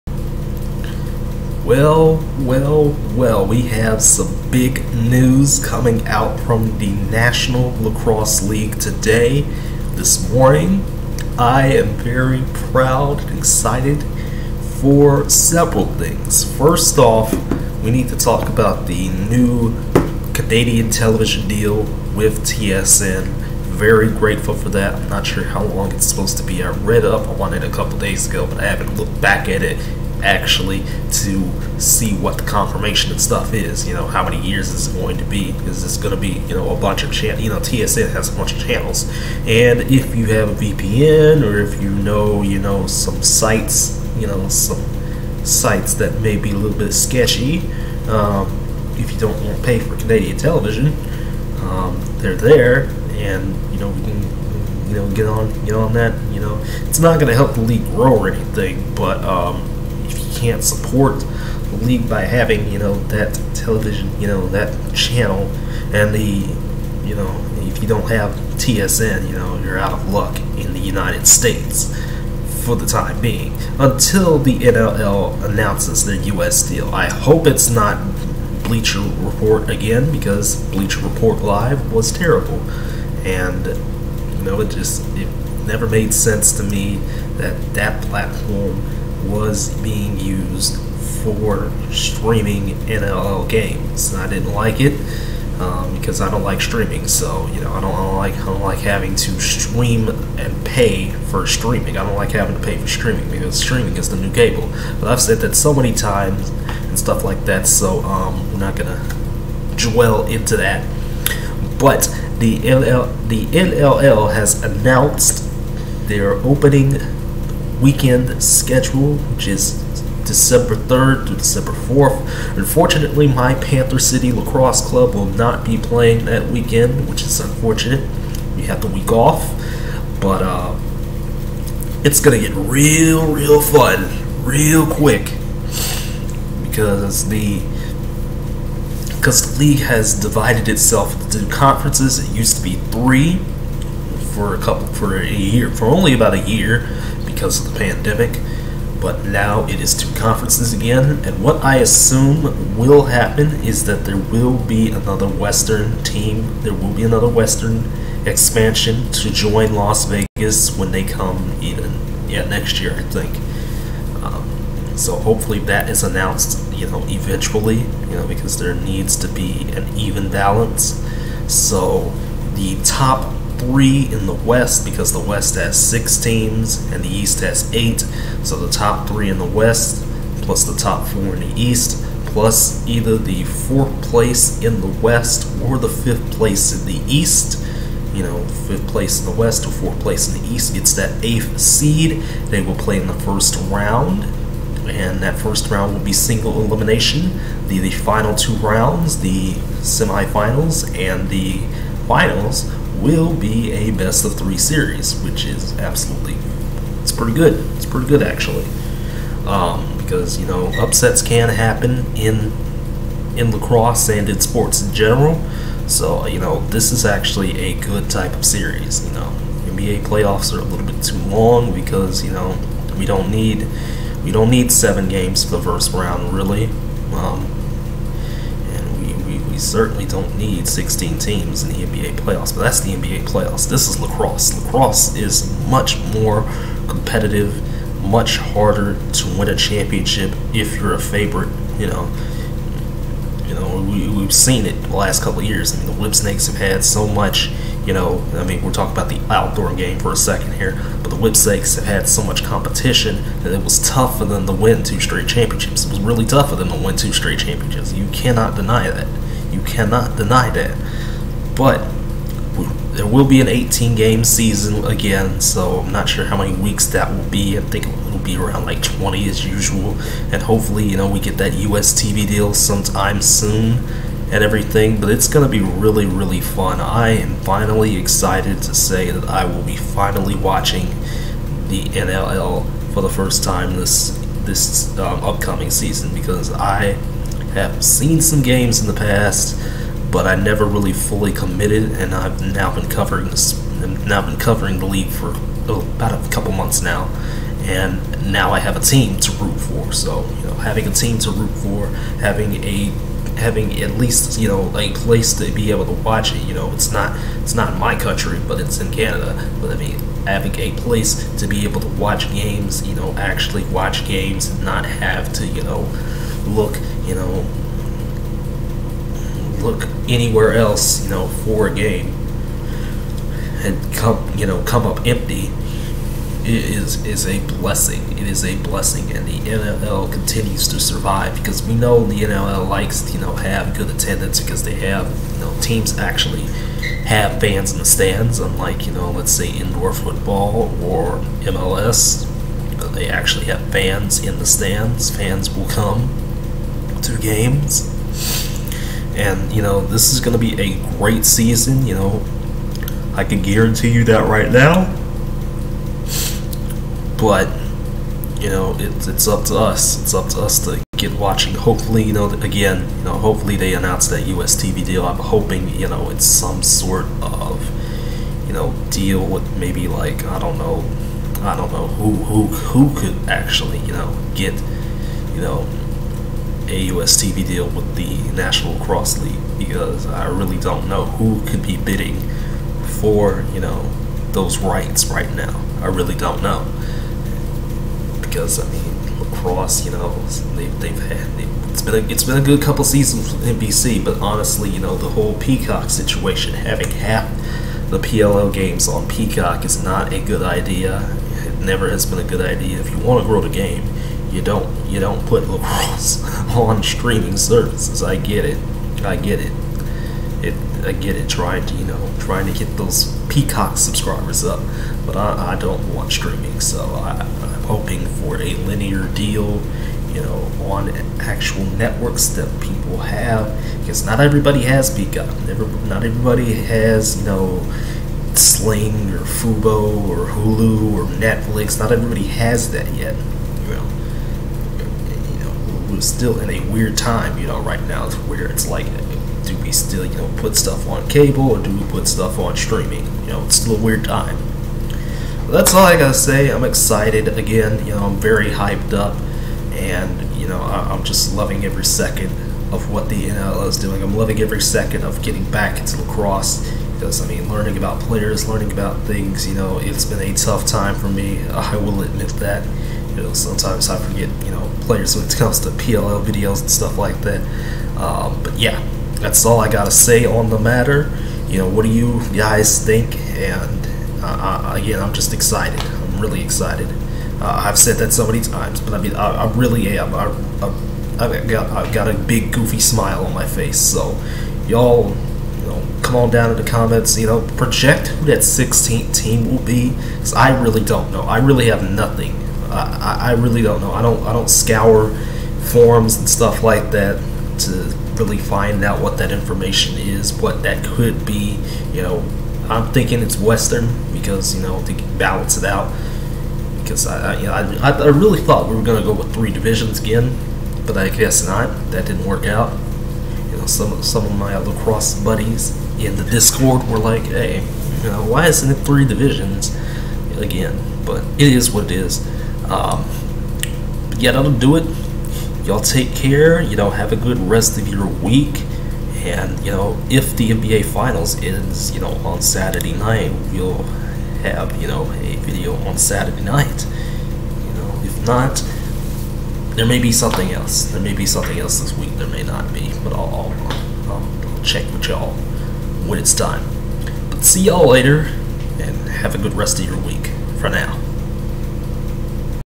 NLL Announces New Canadian TV Deal & Opening Weekend Schedule
HYPE IS REAL